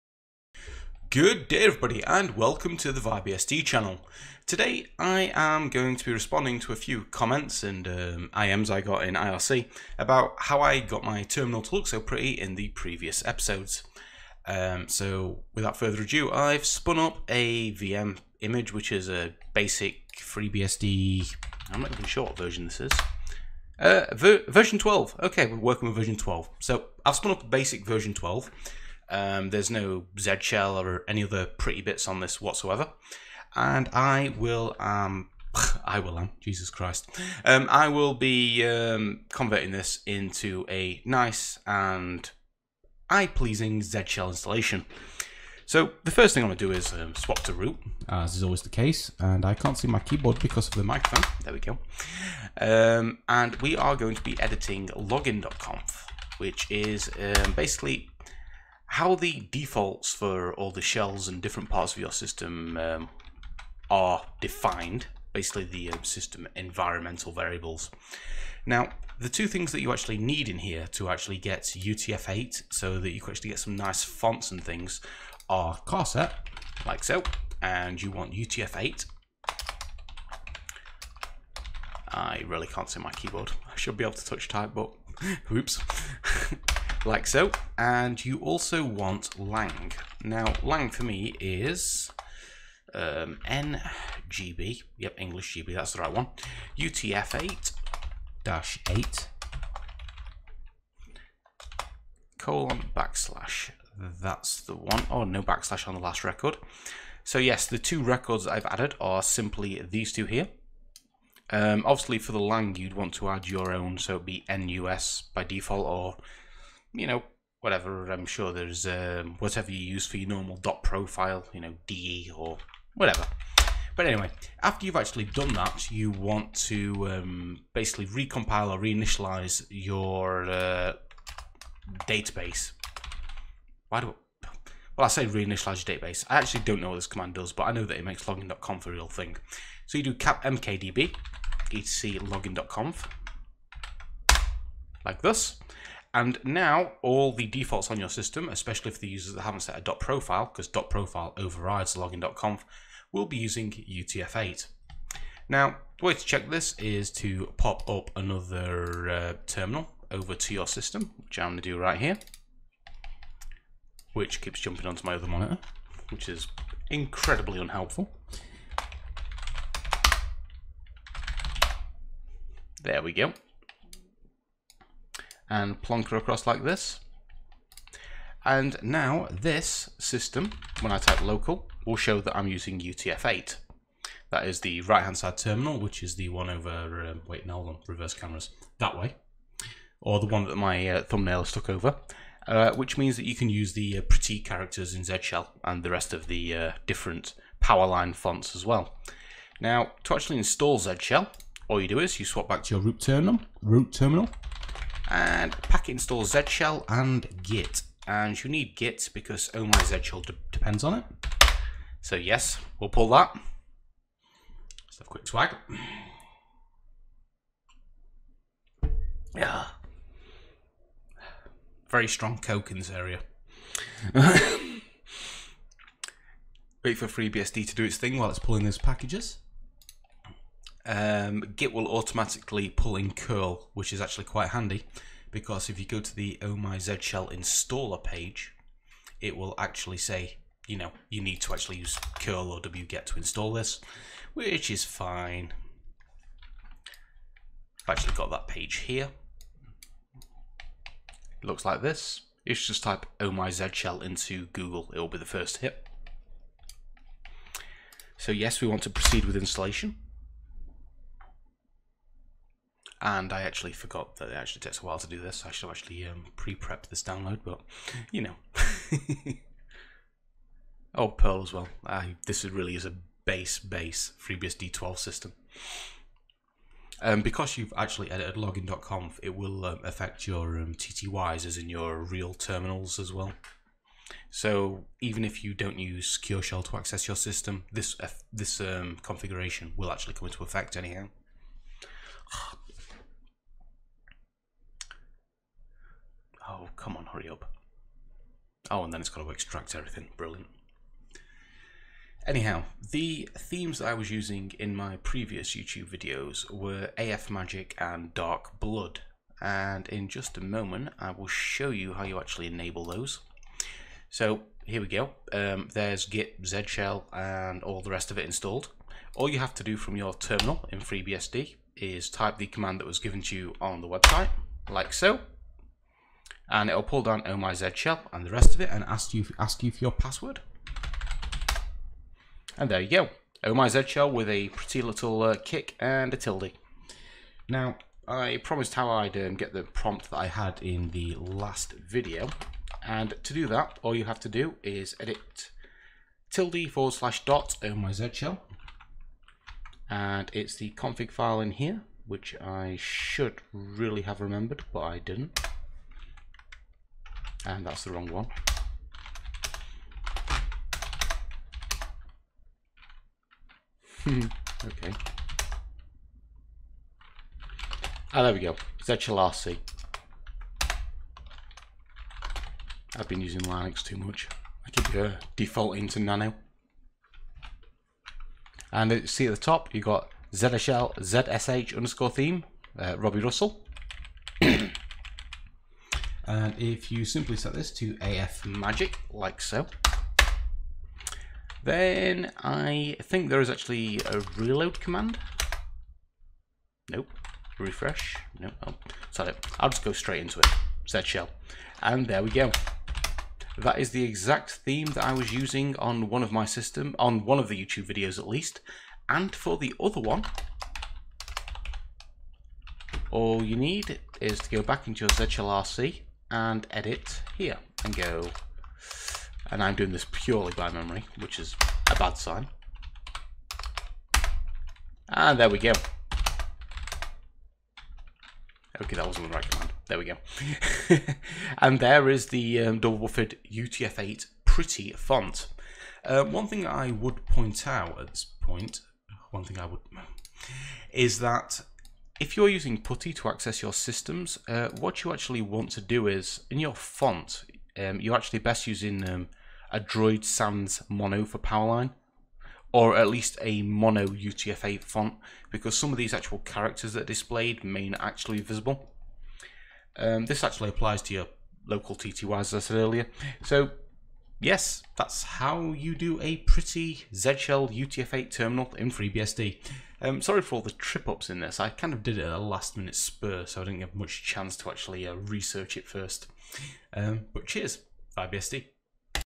Howto: Get pretty things in FreeBSD terminal
A brief run over on howto enable UTF-8 on FreeBSD so that you can use tools like zsh with oh-my-zsh